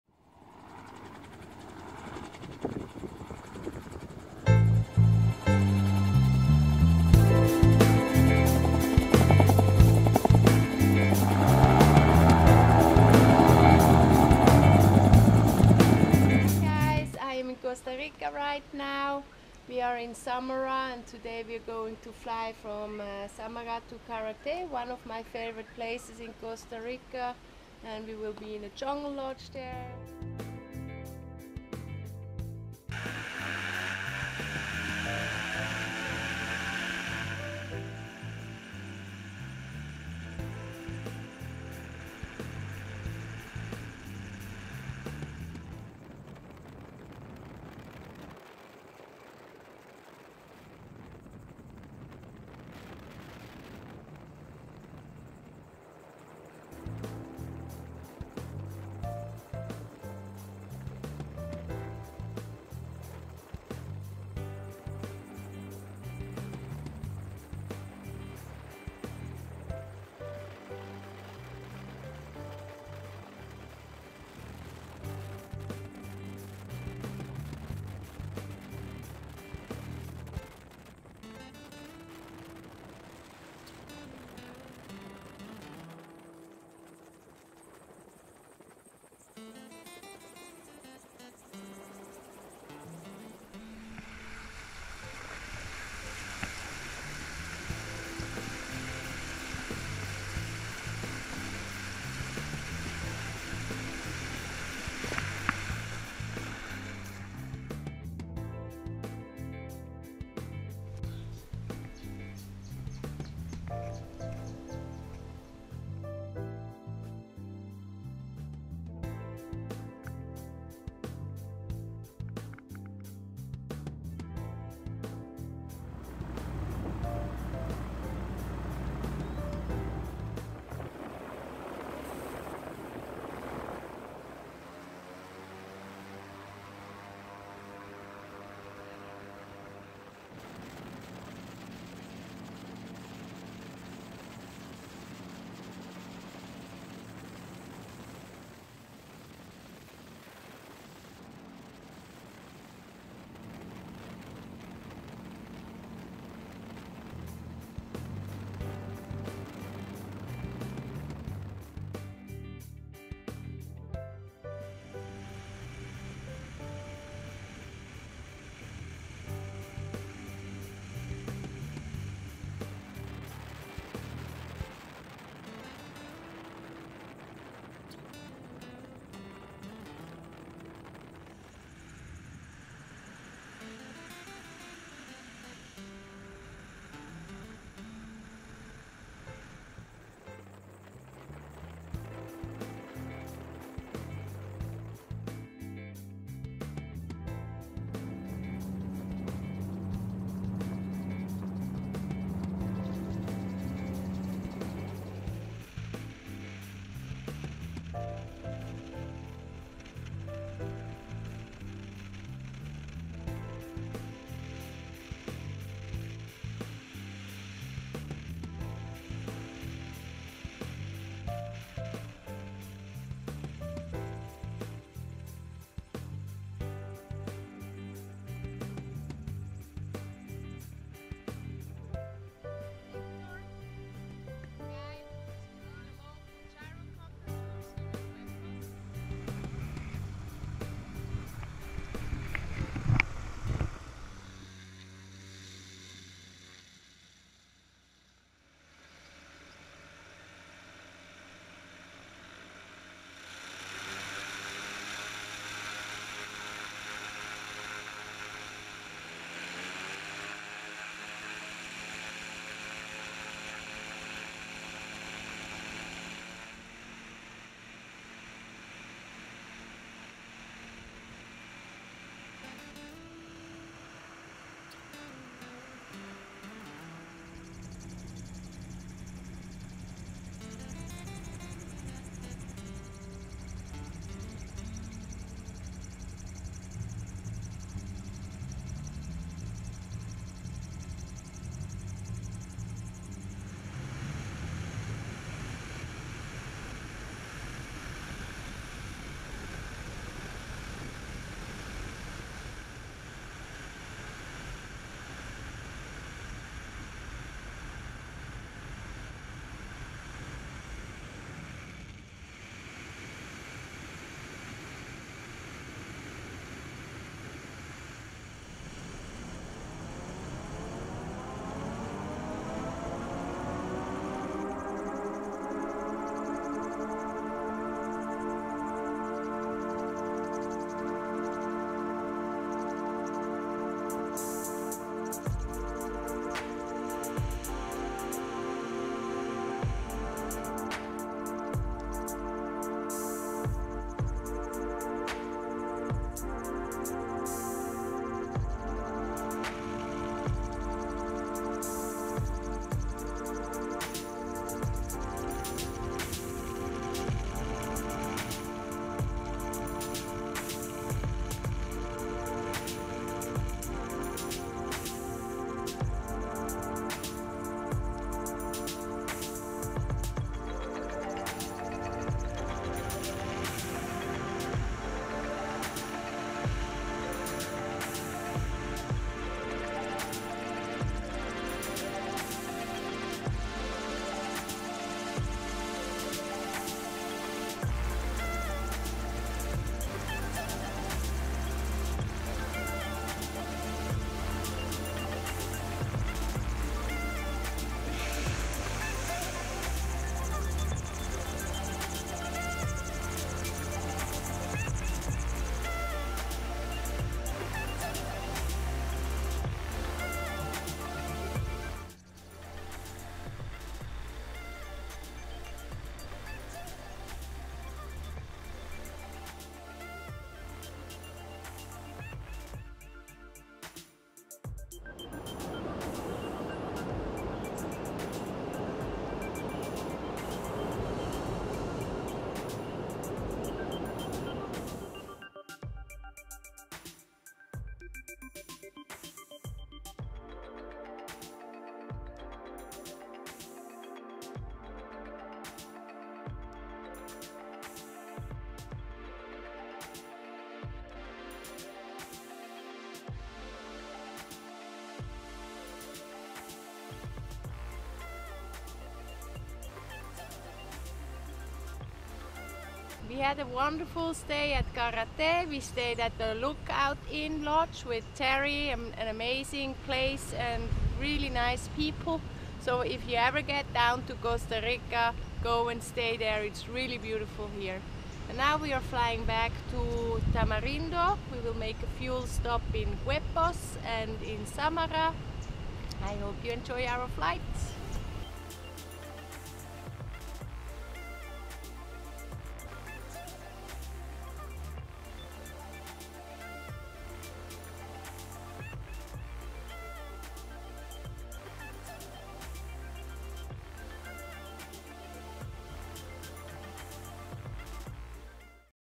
Gyrocopter Girl Flying Costa Rica Carate 2017 03
Enjoy our Trip from Tamarindo to Carate arround 500 km along the Coastline of Costa Rica. See the Volcano from a Gyrocopter

Buying or selling your aircraft? Take a look at the worlds fastest growing platform